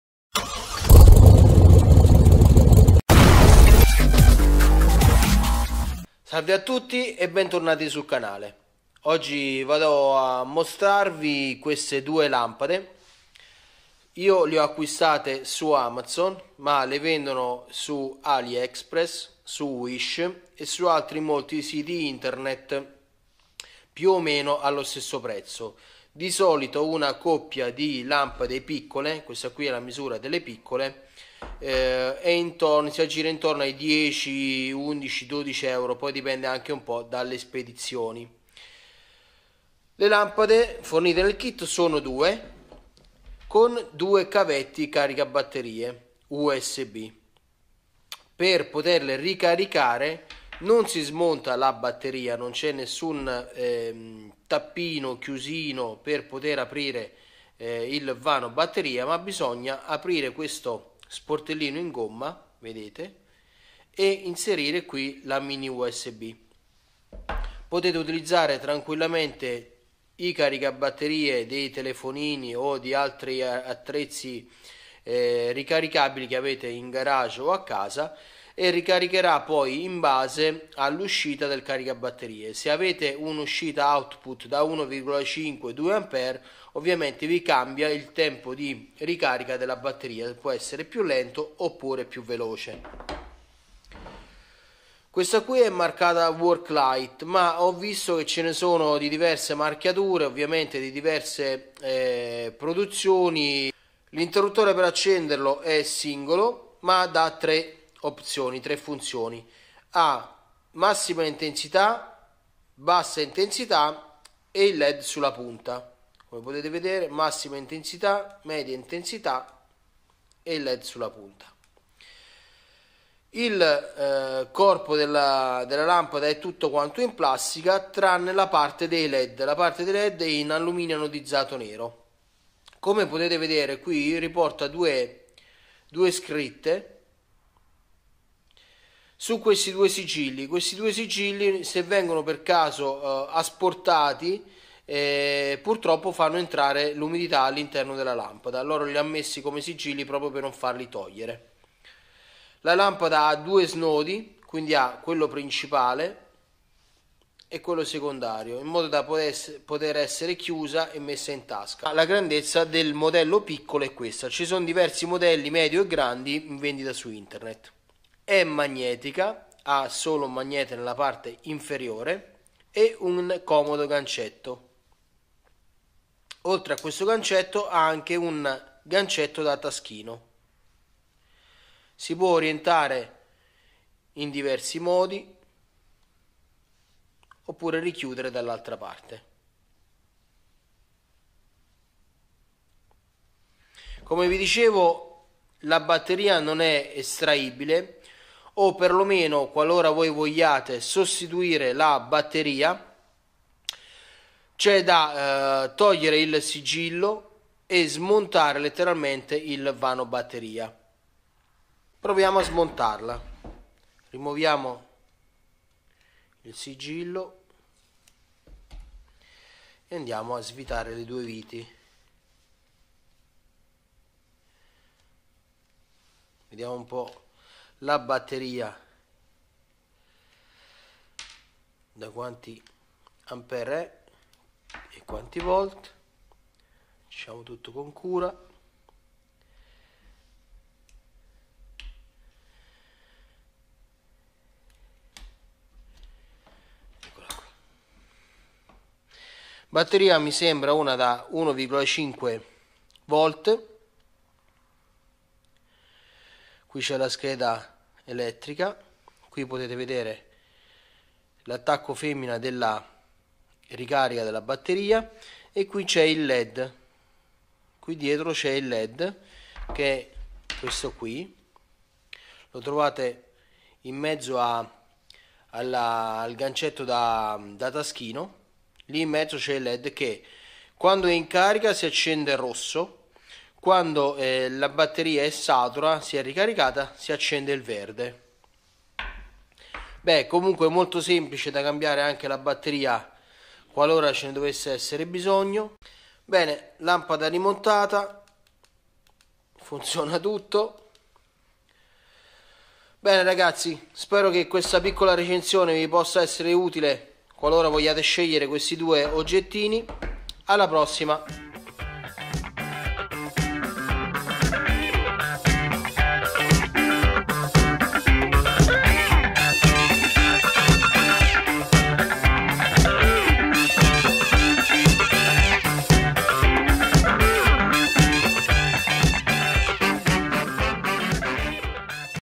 RECENSIONE LAMPADA LED AMAZON
nr.2 lampade da lavoro led con cavetti